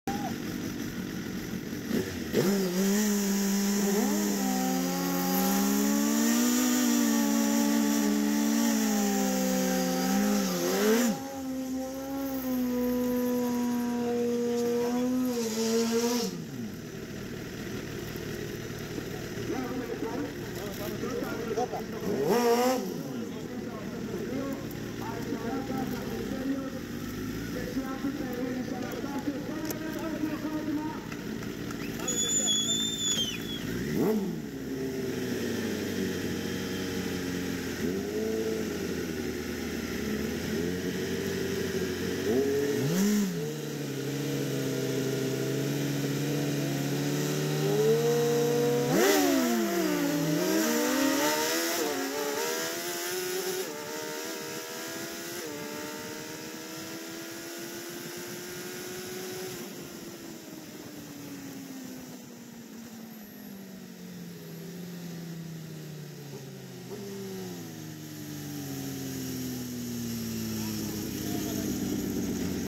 GREEK DRAGSTER LENOE EUROPE 14/04/2019 IMITELIKOS TRUE STREET 9.3 KAZDAGLIS VS PIPERIDI
Gsxr 1300 vs gsxr 1000